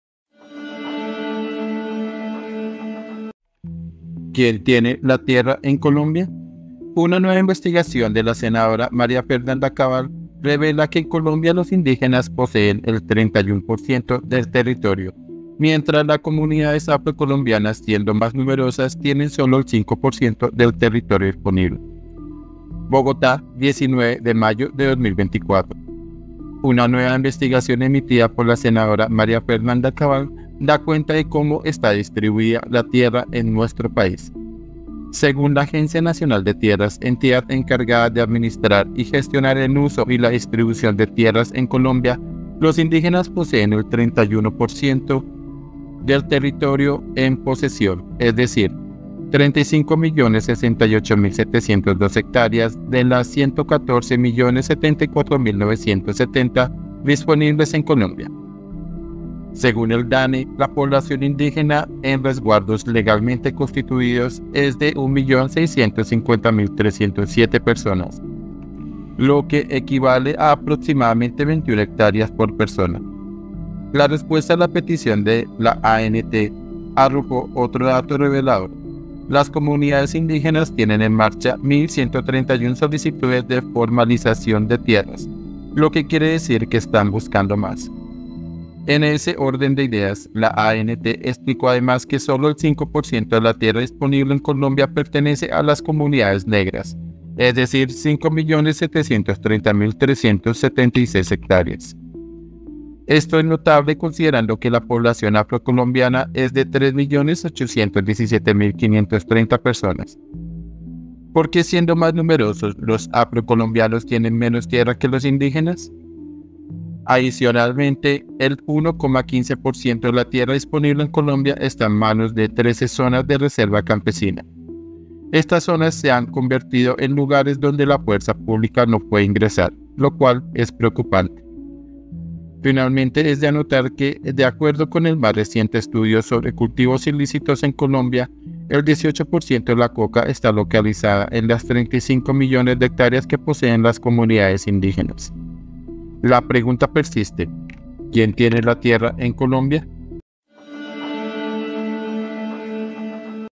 ¿QUIÉN TIENE LA TIERRA EN COLOMBIA? 🚨 DENUNCIA MARÍA FERNANDA CABAL
Boletín ¿Quién tiene la tierra en Colombia? Una nueva investigación de la senadora @MariaFdaCabal revela que en Colombia los indígenas poseen el 31% del territorio.